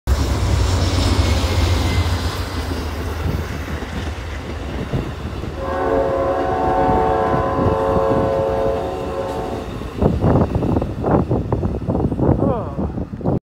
Amtrak’s Empire Builder: Chicago’s Portage Park Neighborhood
Amtrak’s Empire Builder going through Chicago Illinois’s Portage Park Neighborhood. Train is heading for Seattle, Washington on a cold February 19, 2022 day.